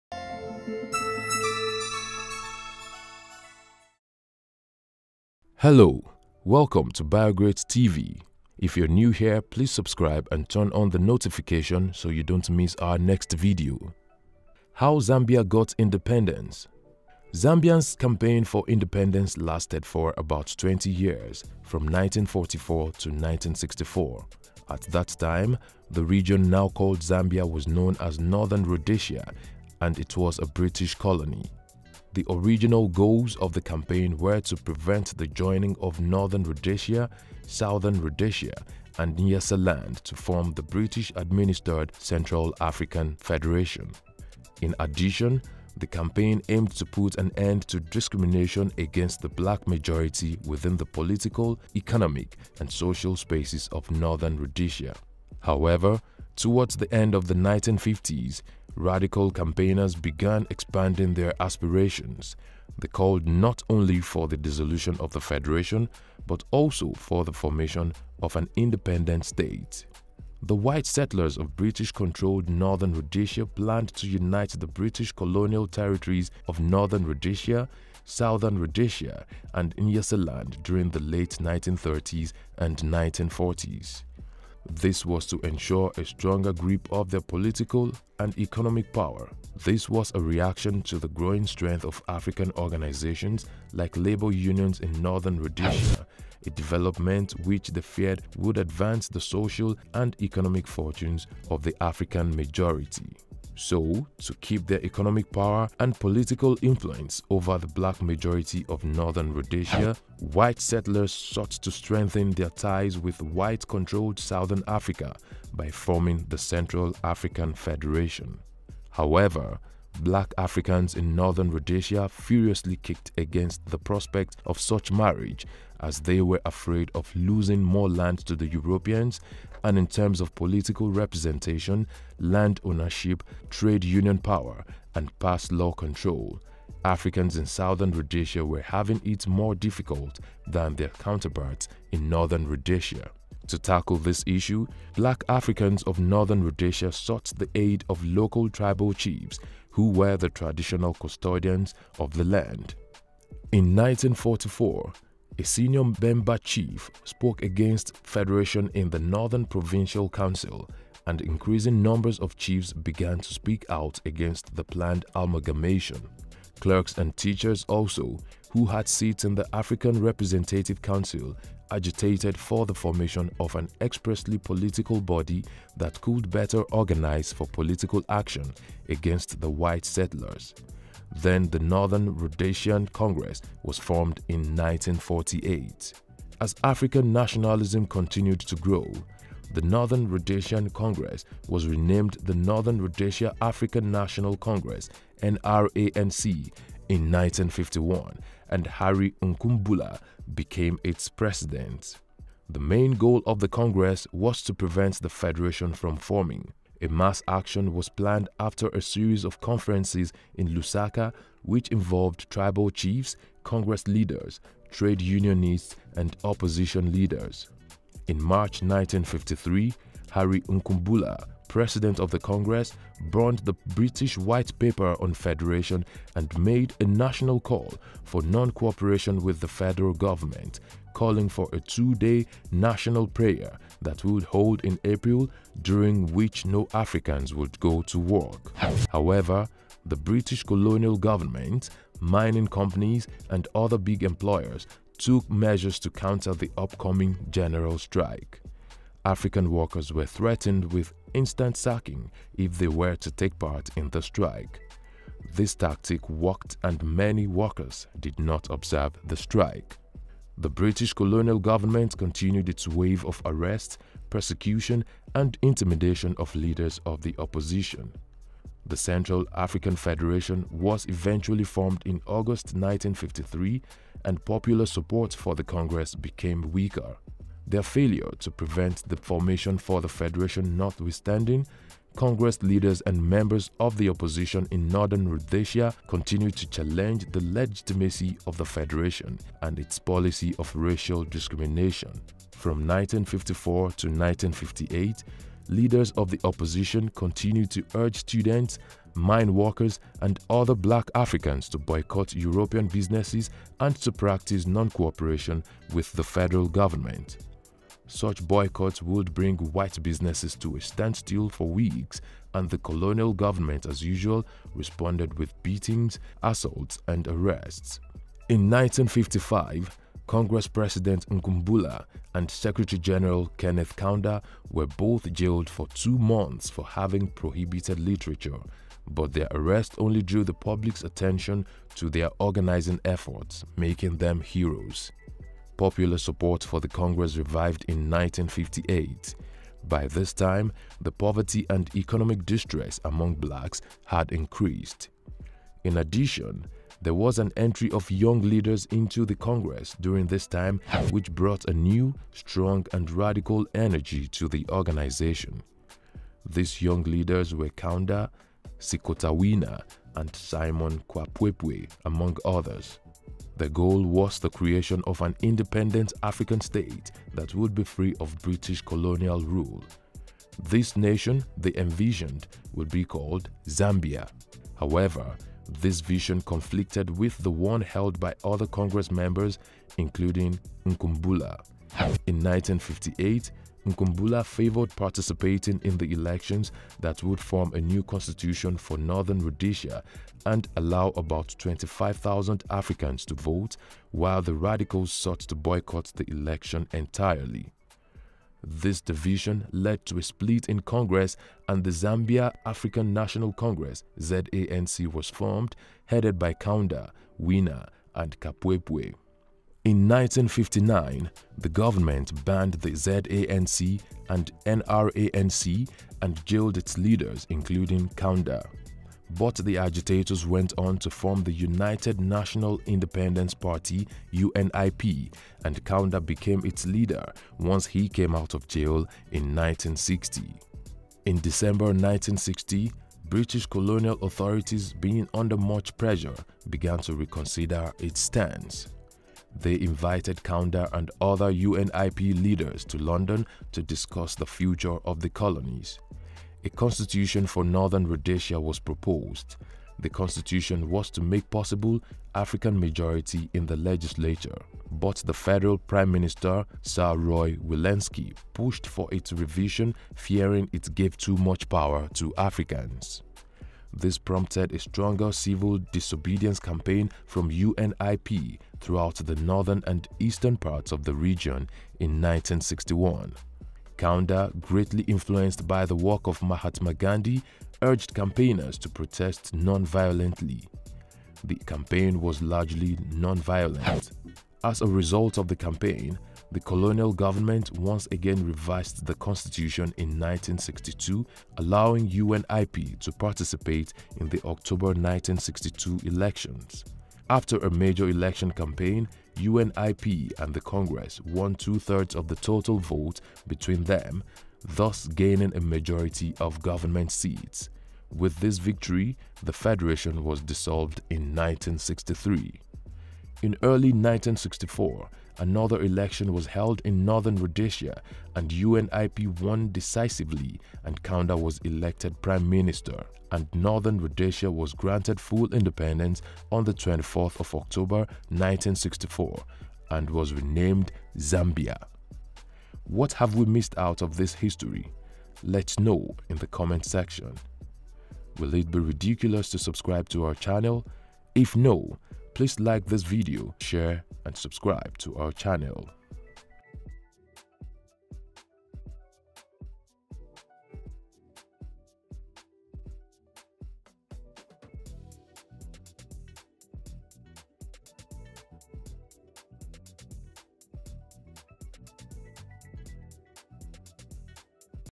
How Zambia got independence
Zambia’s campaign for independence lasted for about 20 years; from 1944 to 1964. At that time the region now called Zambia was known as Northern Rhodesia and it was a British colony. so how did Zambia get freedom? Watch!